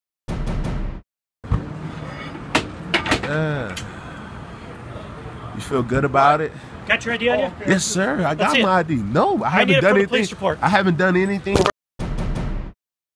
Vikings Tom Johnson- Here's Video Proof Cops Assaulted ME Before Nightclub Arrest | TMZ Sports
Vikings player Tom Johnson says cops left out a big part of the story surrounding his arrest -- which is that officers physically assaulted him before taking him into custody ... and he's now come forward with video.